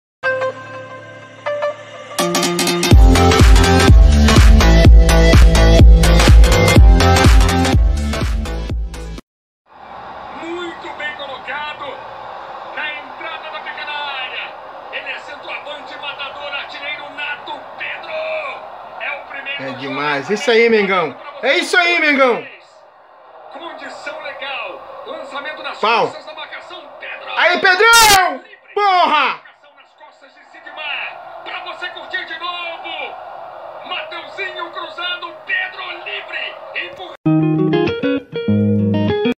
FLAMENGO VENCE GOIÁS POR 1X0 COM PROTESTOS.
FLAMENGO neste sábado 21/05 no Maracanã pela sétima rodada do Brasileirão  2022 venceu o Goiás  por 1x0 gol de Pedro e o que parecia  ficar uma calmaria ao final da partida houve  protestos  por parte da torcida ali presente voltada a Diretoria  com palavras  não  pouco amistosas contra o presidente  Rodolpho Landim.